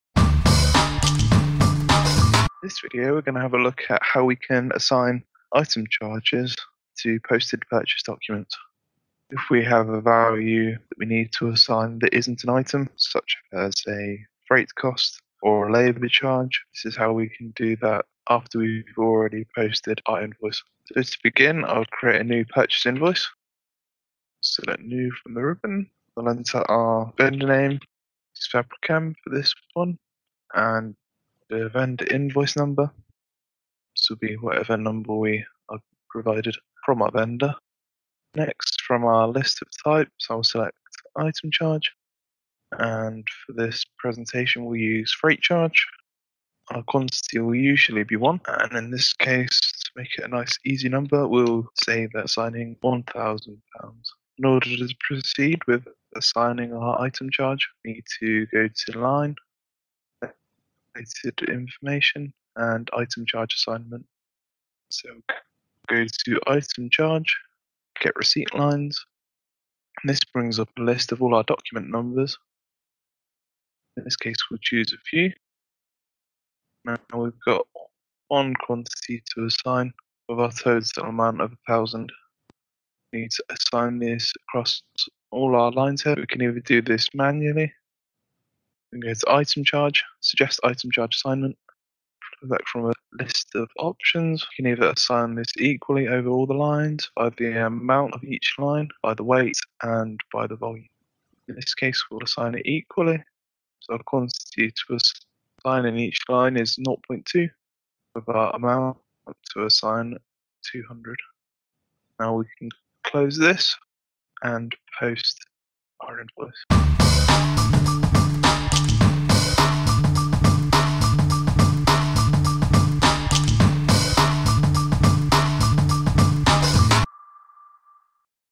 Processing Item Charges on Posted Purchase Documents | Business Central Training Centre
In this video we look at how to process Item Charges on Posted Purchase Documents within Business Central. Part of the Business Central Training Centre from Dynamics Consultants.